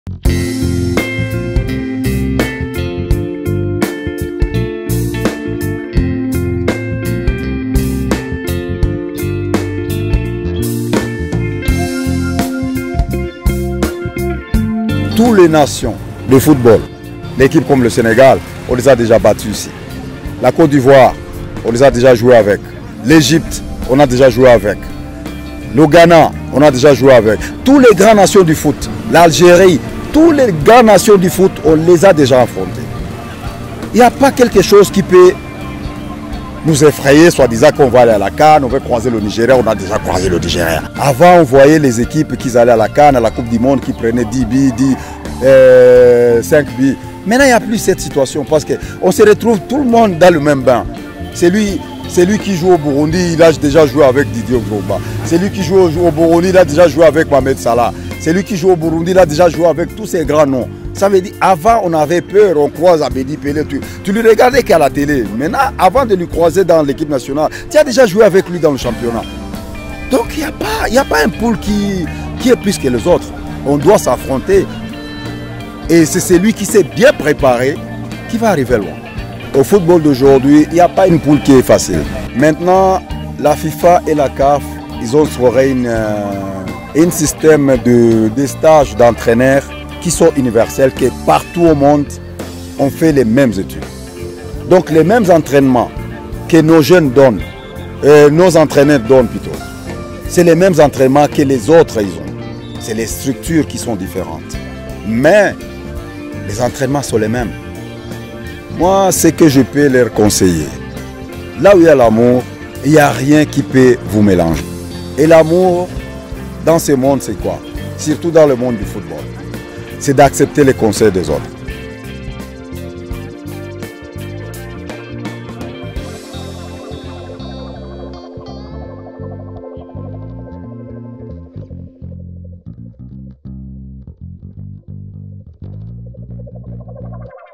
Burundi: Akabanga Djuma Mossi yibira Intamba mu rugamba
Uwahoze ari kizigenza w'intamba mu rugamba Djuma Mossi, arahanura intamba kugira ngo zize zishike kure muri rino higanwa rya CAN 2019